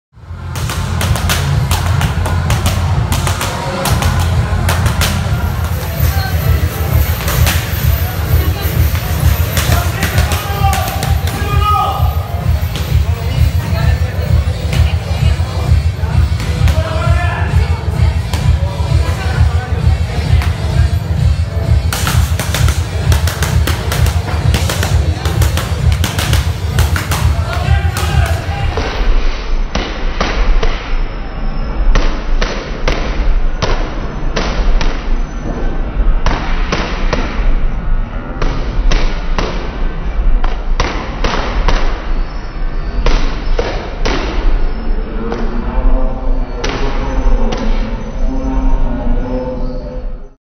ENTRENAMIENTO CON MANOPLAS DE VELOCIDAD | BOXING TRAINING WITH PADS
Entrenamiento de boxeo con manoplas de velocidad, Silvia Torres (Peleadora Profesional) y Norma Torres (Manager Profesional).